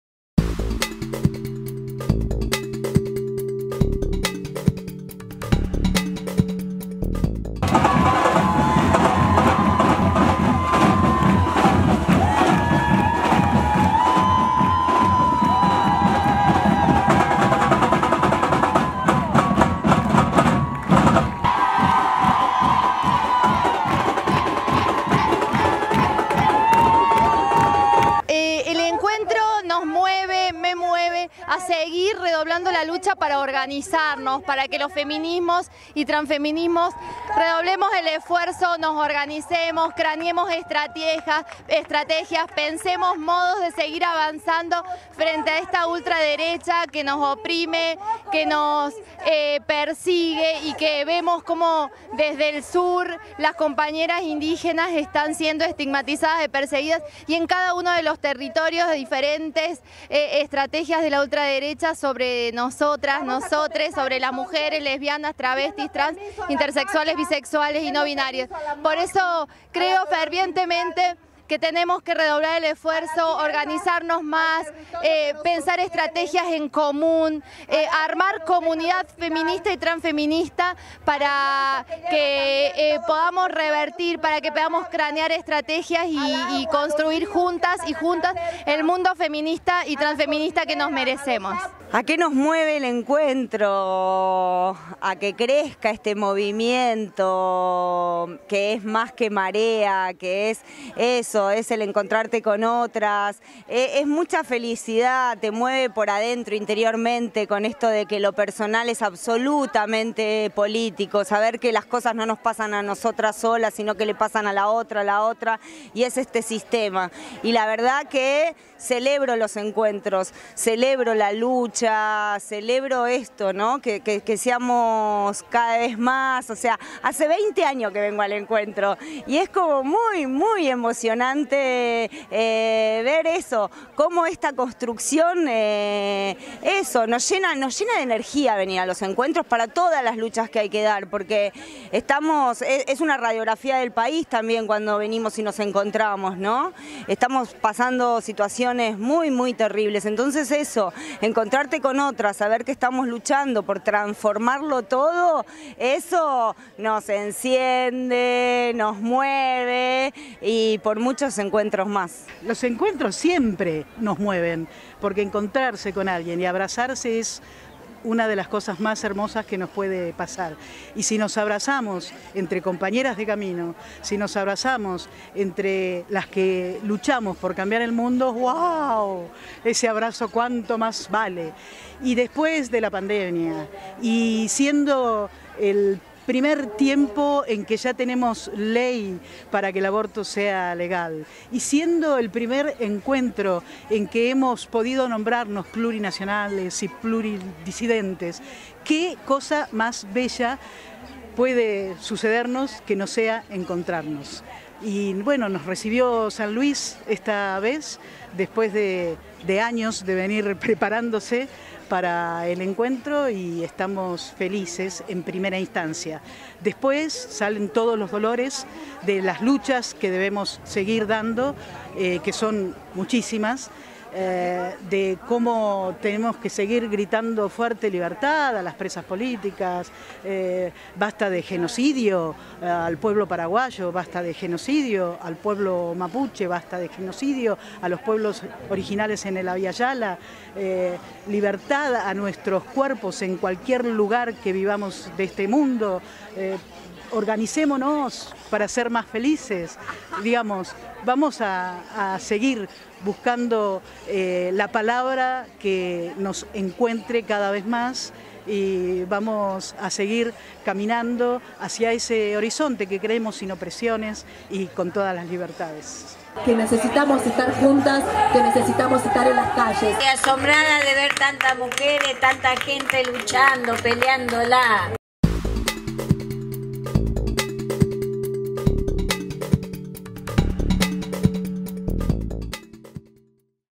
35 Encuentro en San Luis: ¿Qué nos mueve este Encuentro Plurinacional y Pluridisidente?
Los feminismos de todo el país se reunieron en el 35º Encuentro Plurinacional de Mujeres, Lesbianas, Travestis, Trans, Intersexuales, Bisexuales y No Binaries.
Con la consigna de ¿qué nos mueve el encuentro? compartimos testimonios que nos cuentan cuáles son las emociones, reclamos y luchas que llevan a los feminismos a participar de estos espacios.